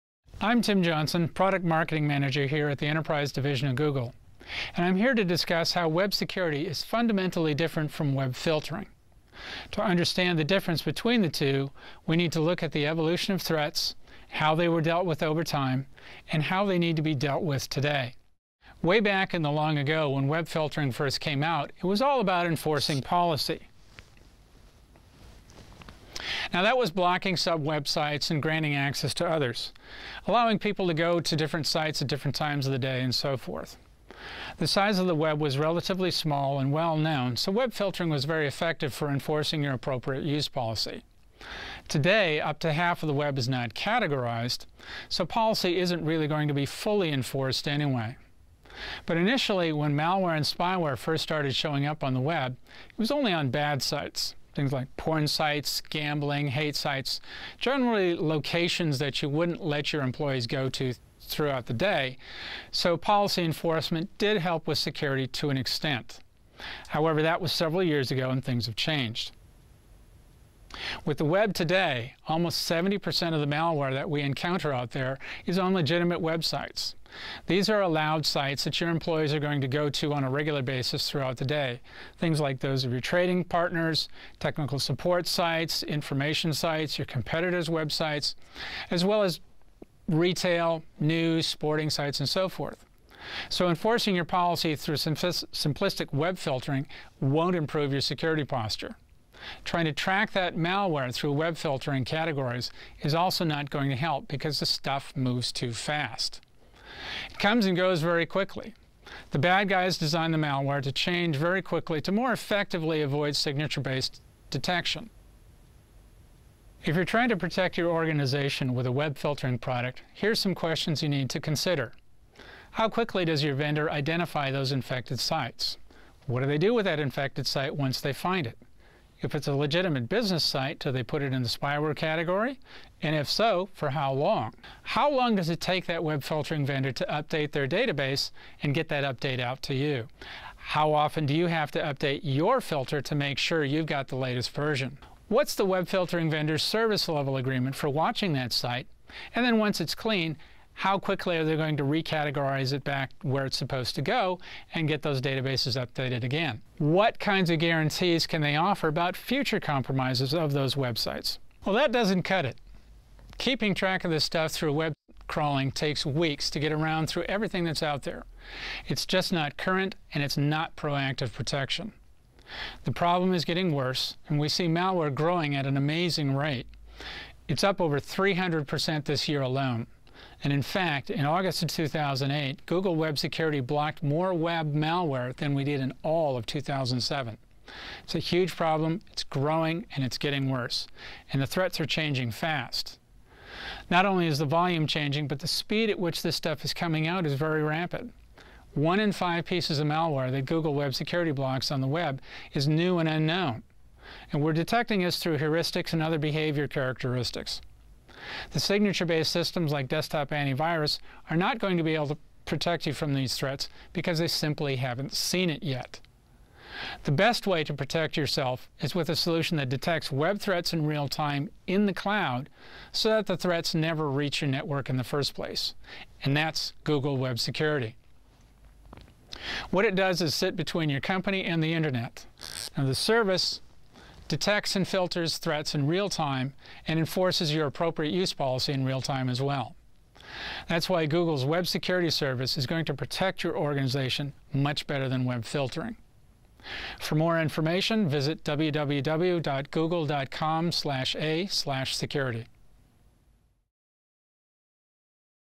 Google Web Security
- Google Web Security protects organizations of all sizes against web malware attacks and enables the safe, productive use of the web, without incurring hardware, upfront capital, or IT management costs. It protects all users, whether they are connected directly to the corporate network or working remotely, at a hotel or in an airport.